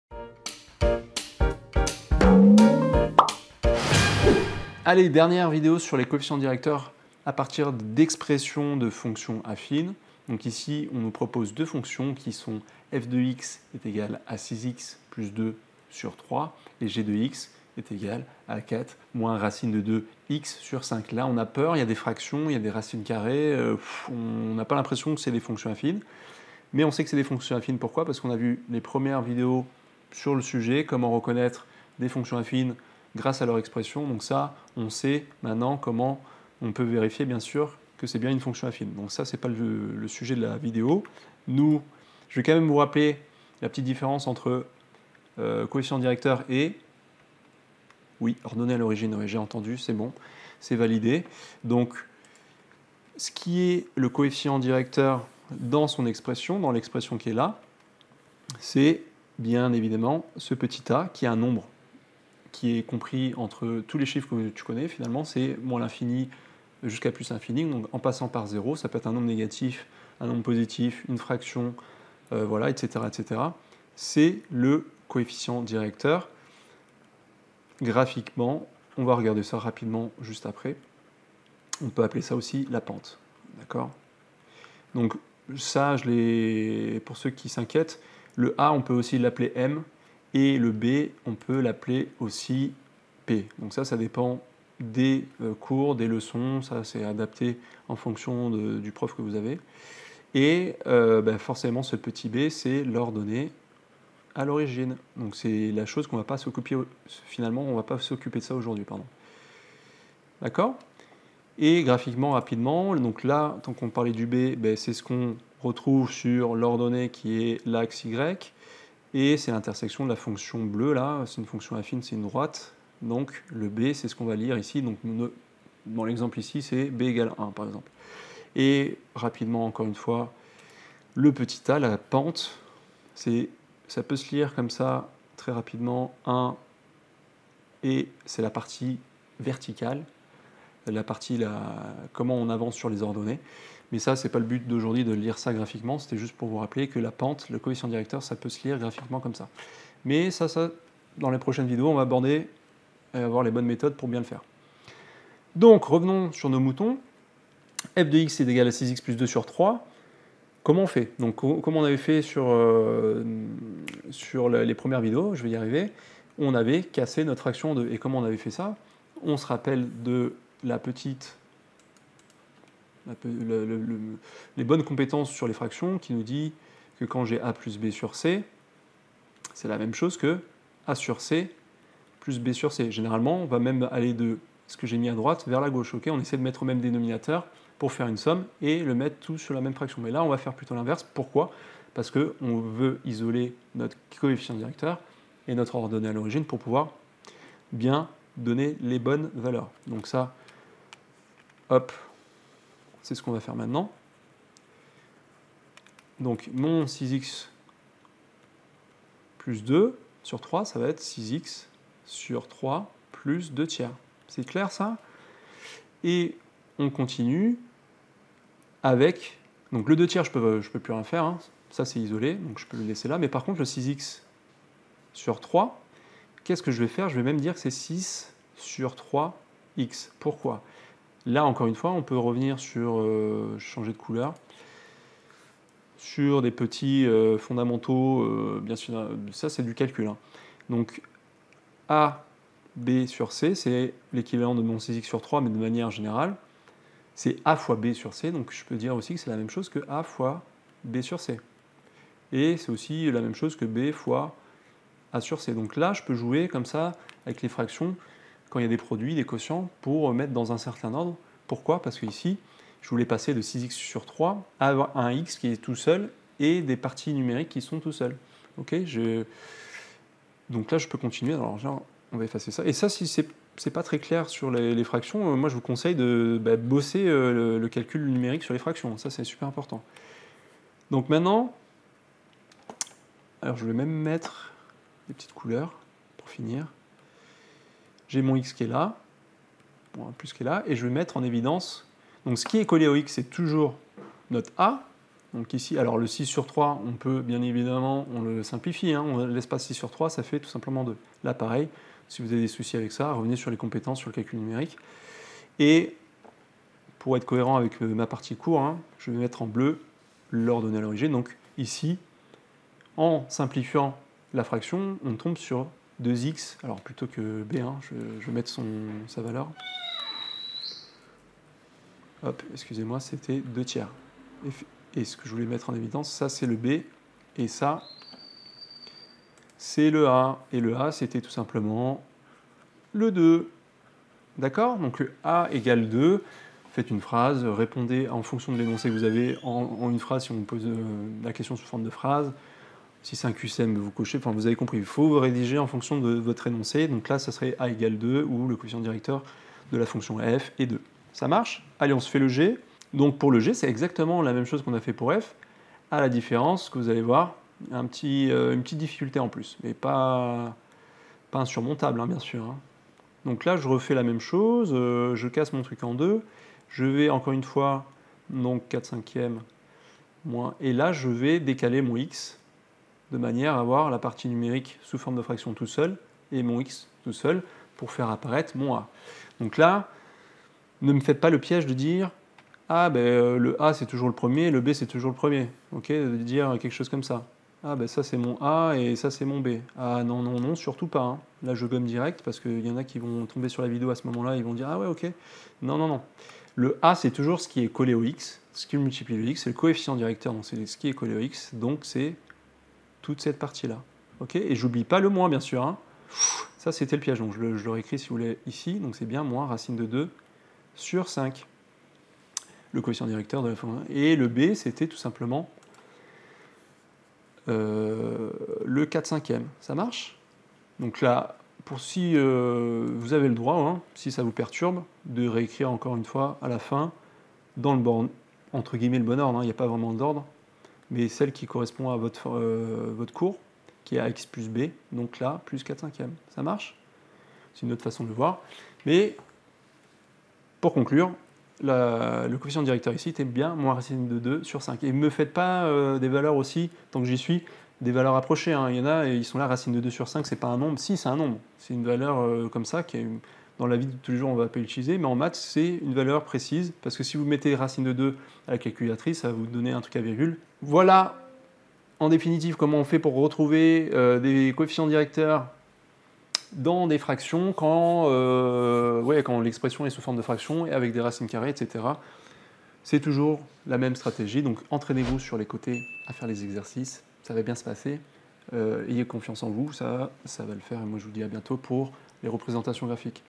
Trouver le COEFFICIENT DIRECTEUR dans une EXPRESSION #3 - Exercice Corrigé - Maths Troisième
mathématiques troisième exercicecorrigé Comment retrouver un coefficient directeur d'une fonction affine à partir de son expression algébrique ?

L'expression d'une fonction affine est : f(x) = ax + b (ou f(x) = mx + p).

Le coefficient directeur est la constante "a", aussi appelé "pente".

Rendez-vous sur galilee.ac pour faire des exercices de maths en ligne avec corrections et vidéos à l'appui.